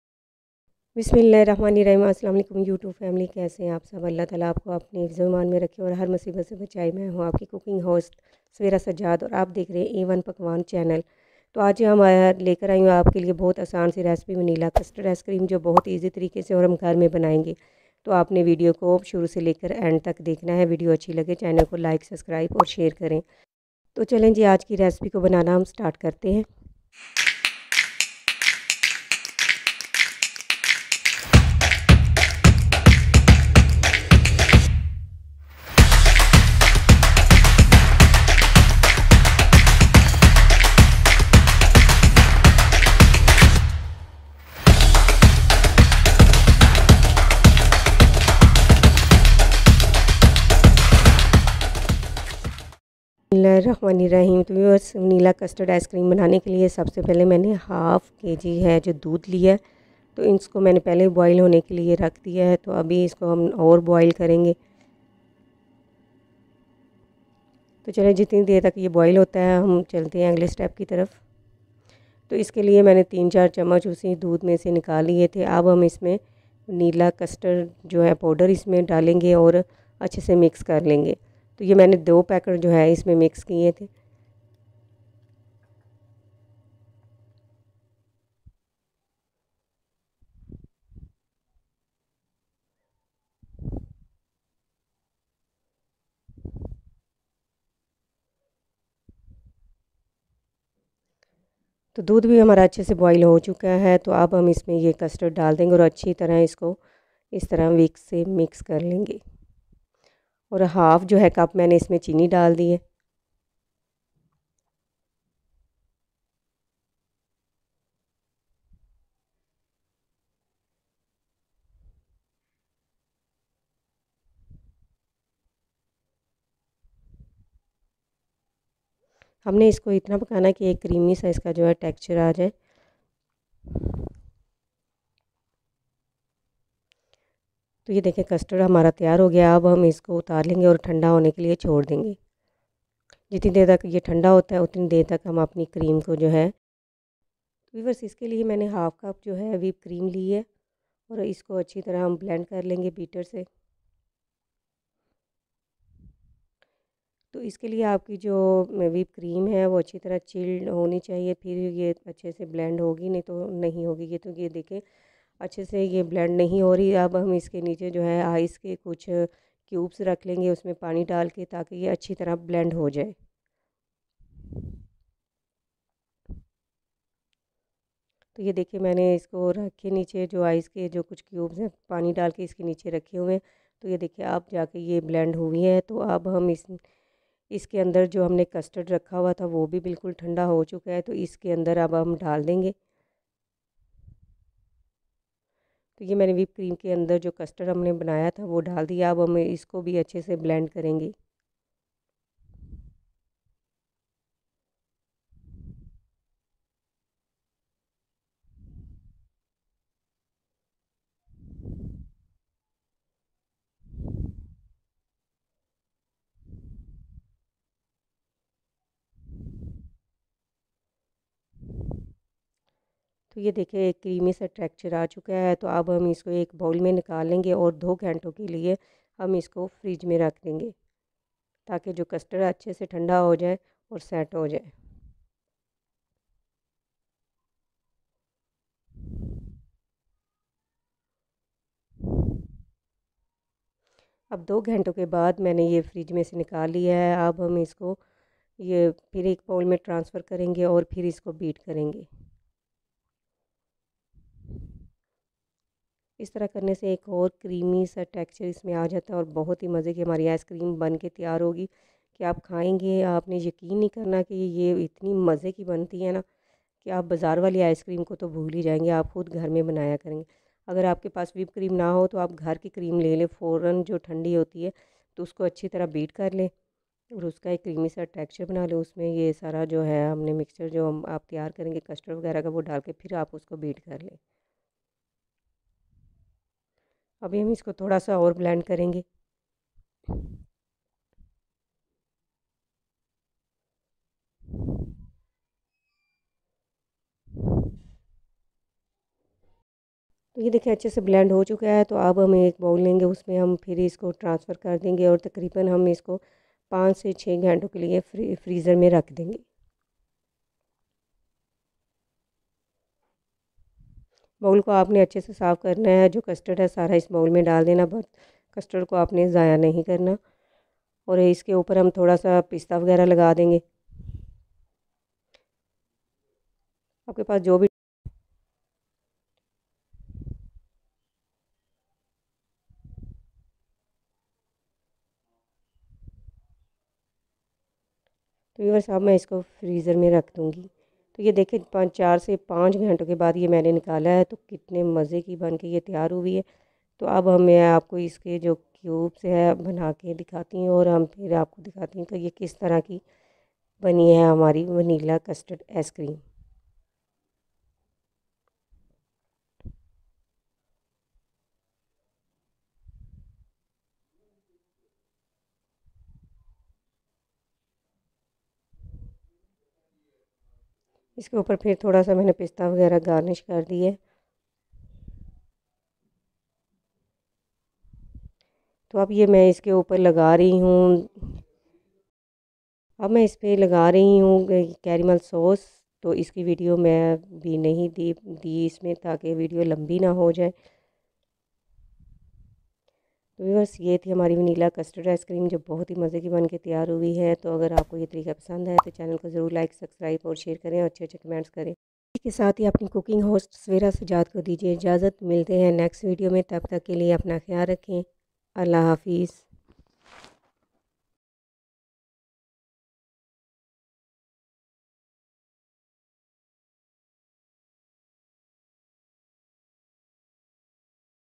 Custard Ice Cream Recipe | Vanilla Ice Cream With A One Pakwan | Homemade Ice Cream
Today i have made a special recipe. Vanilla ice cream is the tasty recipe. You can make ice cream with half litre milk. Vanilla Custard ice-cream is very delicious to eat.